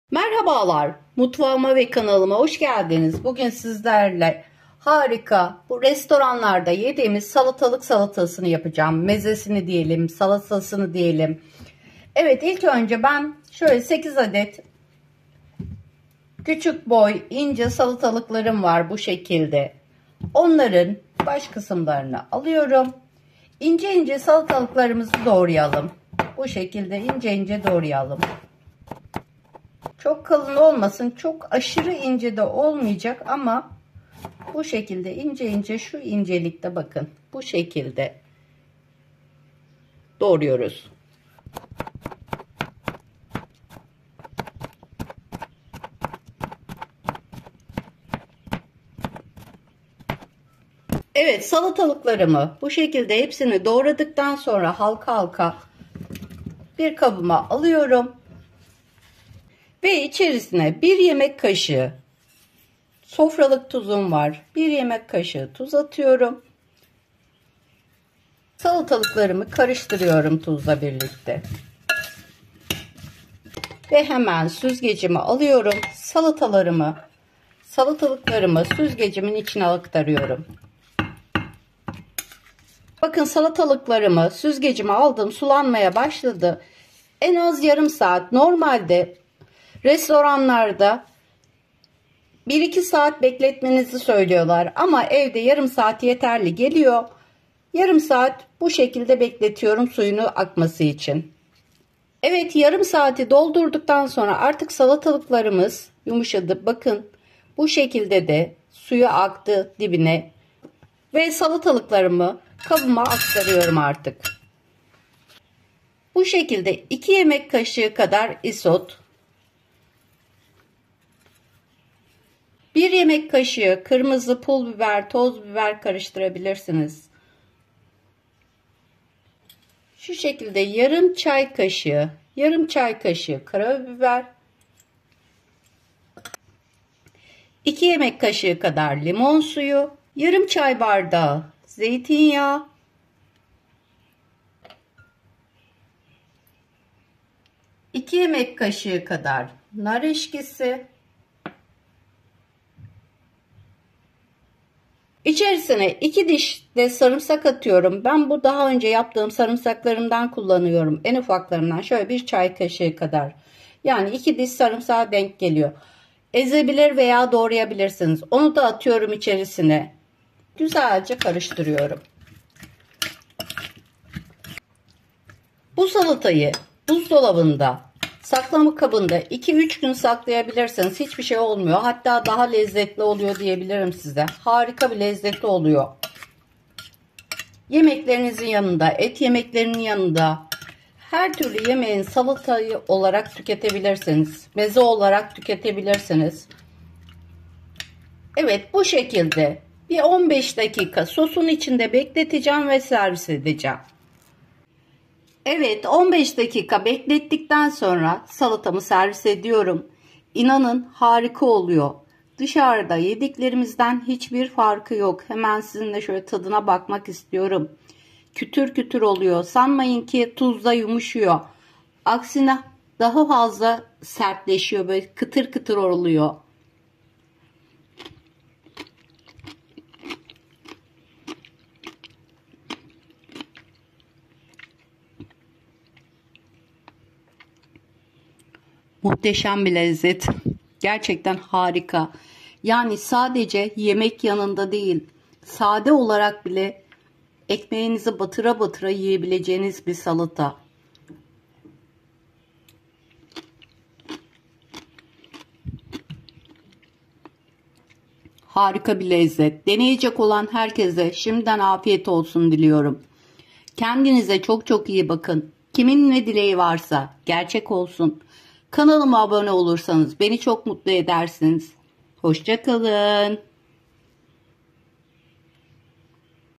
SALATALIK SALATASI Nasıl Yapılır ? RESTORAN SALATASI TARİFİ
LOKANTALARDA MİNİCİK KAPLARDA GELEN O MUHTEŞEM SALATA TARİFİ SİZLERLE 👌TAM ÖLÇÜSÜ İLE KOLAY SALATA TARİFİ ✅

MALZEMELER
8 tane salatalık ( çekirdeksiz Çengelköy salatalık)
1 yemek kaşığı tuz (salatalığın suyunu salması için) rahatsızlık duyanlar tuz miktarını azaltabilir
2 diş sarımsak
2 yemek kaşığı dolusu limon suyu
2 yemek kaşığı dolusu nar ekşisi
2 yemek kaşığı İSOT
1 yemek kaşığı pul biber
1/2 çay kaşığı karabiber

TÜM DİLLERDE ALT YAZI MEVCUTTUR LÜTFEN KENDİ DİLİNİZDE AKTİF EDİNİZ ♥️♥️